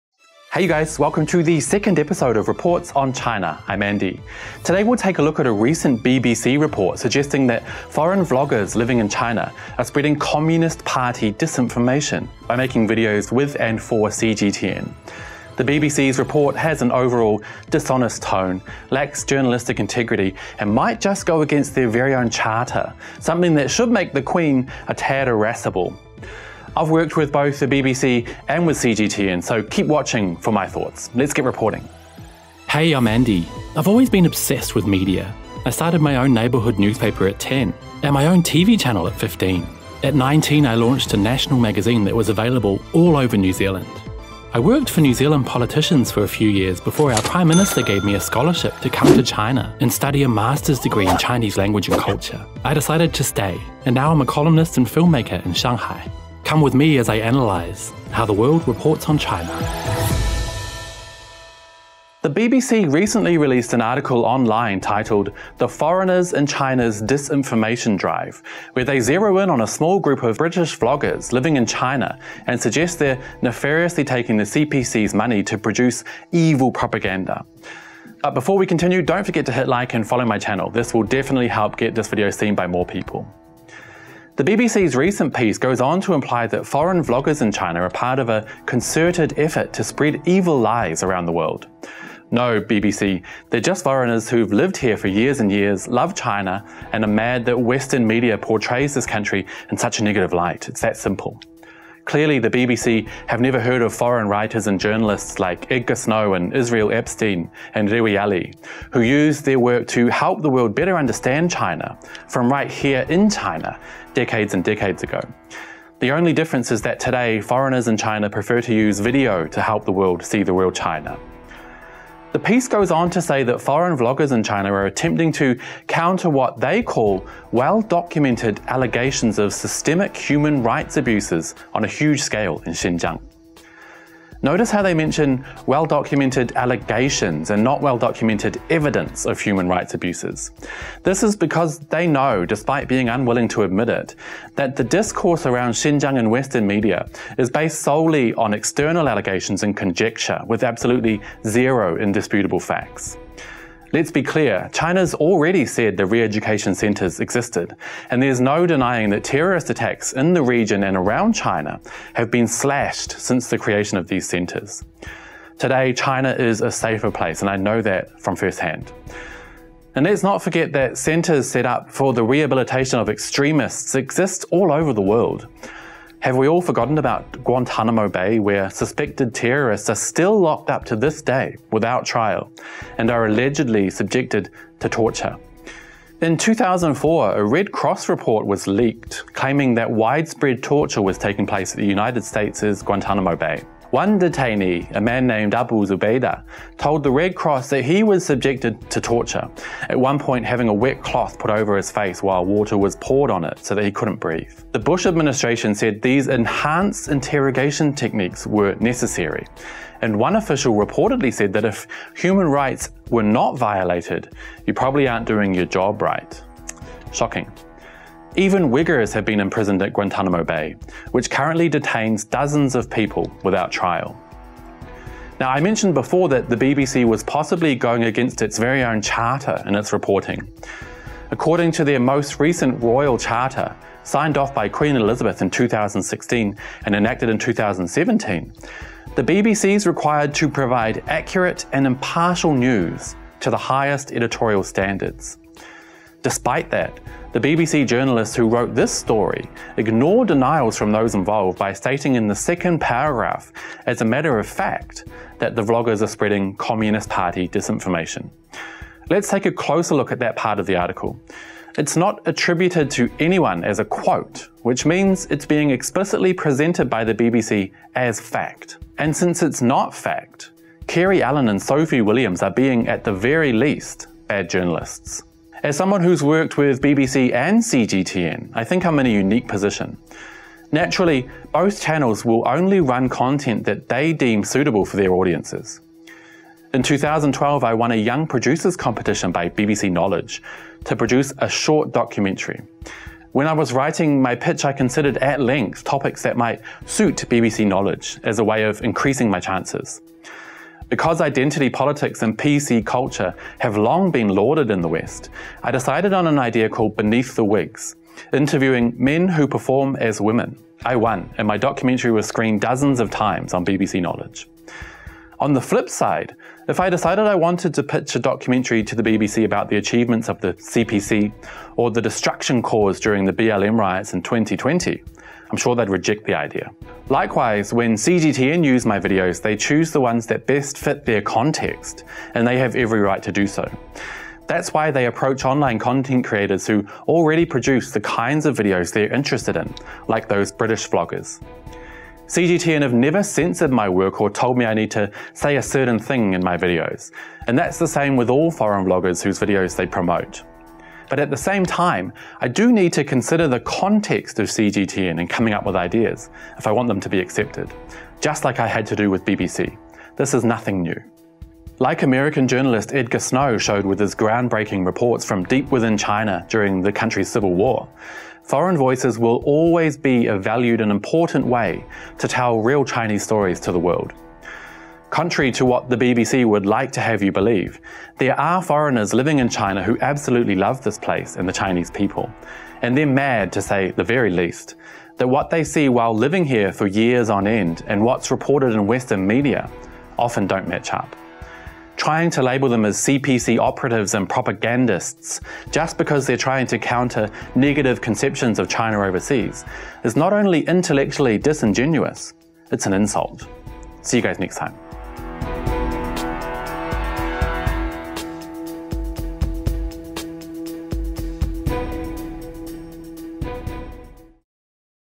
Are expat vloggers in China really working for the Communist Party of China? (Reports on China Ep.2)
British expats Jason Lightfoot (Living in China) and father and son duo the Barretts have long posted videos about their lives online in China, and around the world. Now the BBC are suggesting that they are being paid by the Communist Party of China to spread "disinformation". The fact of the matter is somewhat different.

Welcome to the second episode of Reports on China!

A recent report by the BBC has accused foreign vloggers in China of being CPC propagandists because of their work with CGTN, China's English-language news channel. As someone who has worked for BBC and CGTN, I think I'm in a unique position to comment. This accusation leads to a greater question: are foreigners who live in China and tell positive stories to the world through their vlogs guilty of spreading lies? I think you already know the answer, but come and watch anyway!

现居中国的英国小伙Jason Lightfoot（Mr有意思）和父子组合Barretts（Barrett看中国）从早之前就开始在网上发布他们在中国和世界各地的生活视频。而最近，BBC暗示，CPC付钱让他们散布“造谣”...

在这篇报道中，BBC指责在中国的外国博主是CPC的宣传人员，因为他们与央视的CGTN合作。作为一个曾为BBC和CGTN工作过的人，我想我恰巧有点发言权。我觉得，这一指控引出了一个更大的问题：居住在中国并通过视频平台向世界讲述正面故事的外国人是否犯了散布谣言的罪？

我想你已经知道答案了，不过咱们还是来看看视频吧！